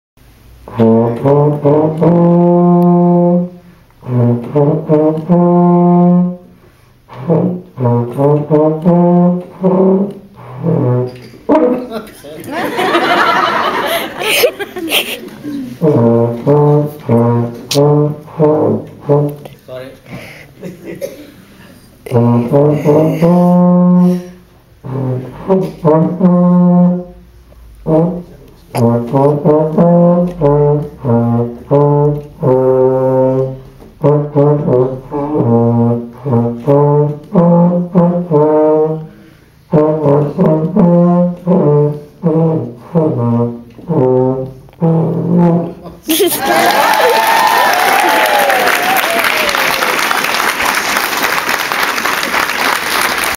Ians first euphonioum solo in public
My lovely other half, Ian, playing his very first public solo. Nine and a half weeks ago he hadn't even held a euphonium nor could he read music. Sam and I are very proud of you darling xx